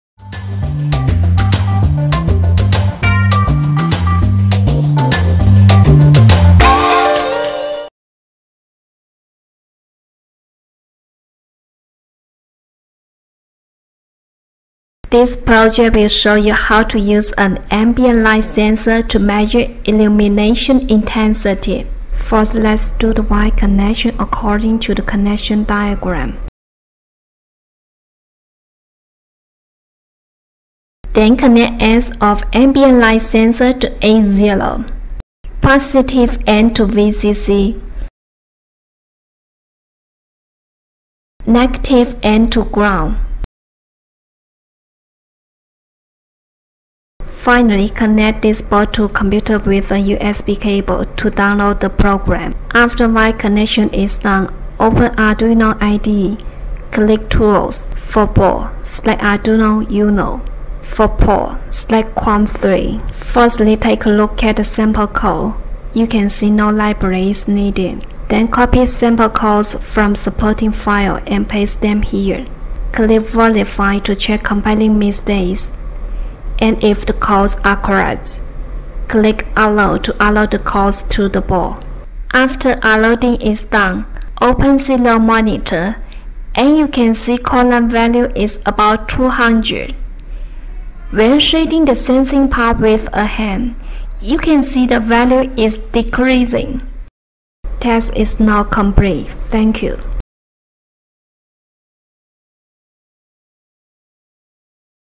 Module Project 8: TEMT6000 Ambient Light Sensor
ks0190 keyestudio Environment Monitoring Kit
This project will show you how to use an ambient light sensor to measure the illumination intensity.